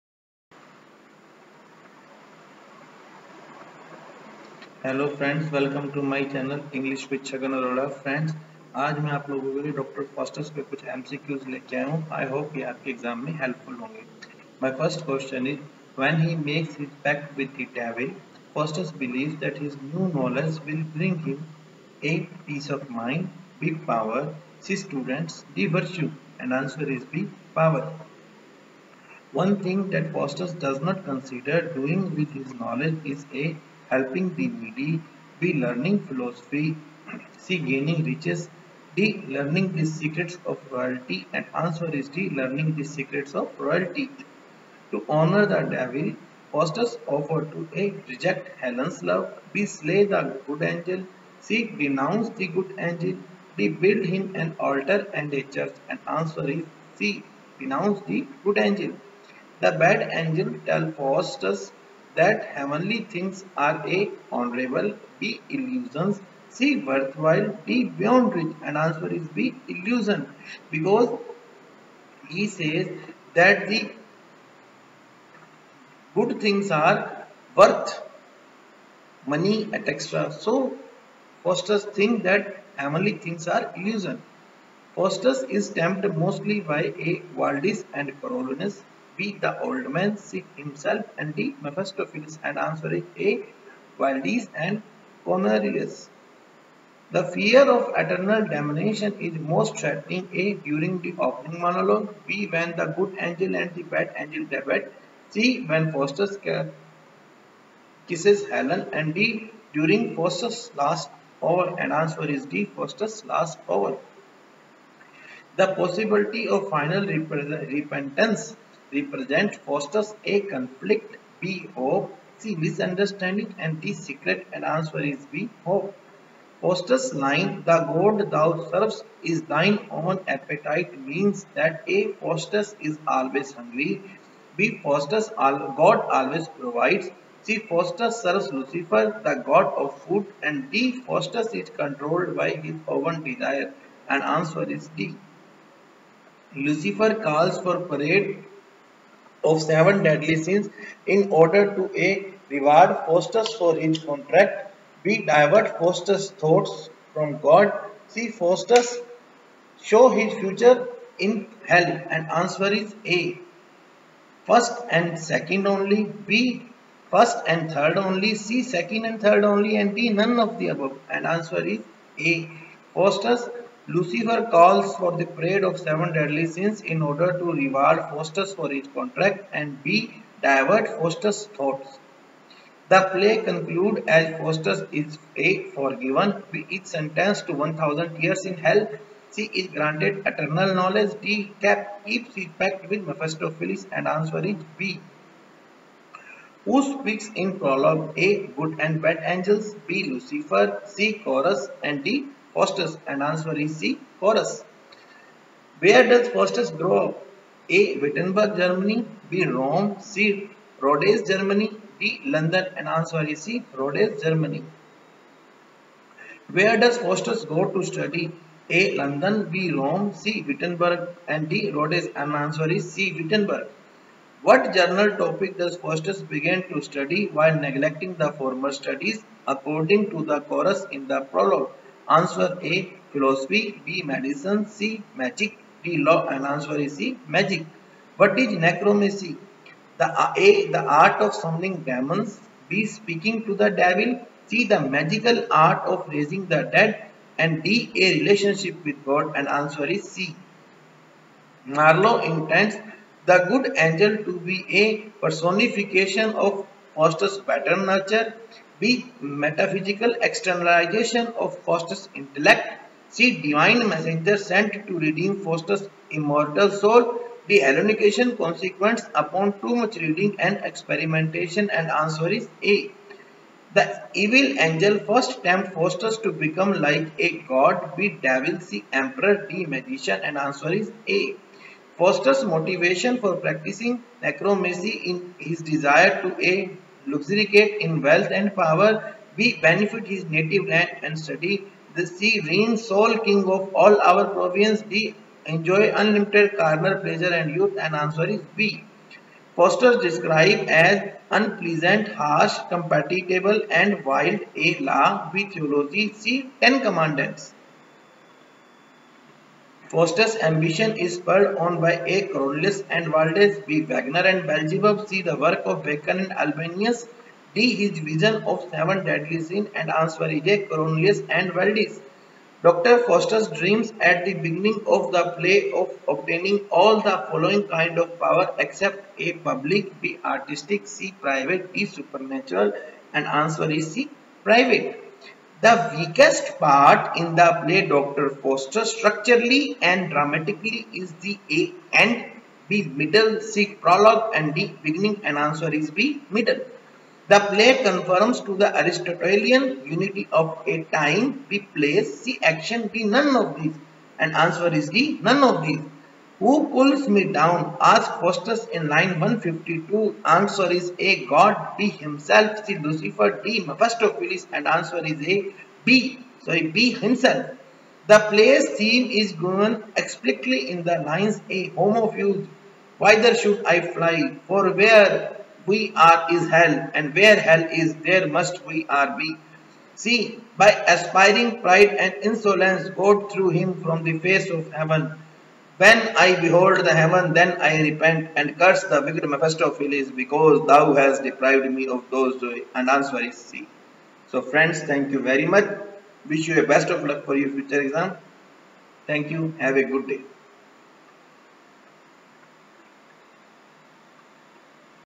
Some most Important expected MCQs on Dr.Faustus By Christopher Marlowe
Some most Important expected MCQs  on  Dr.Faustus By Christopher Marlowe from UGC NET, SLET ,Lt Grade  point of view

Thanx a lot for your support. Keep supporting.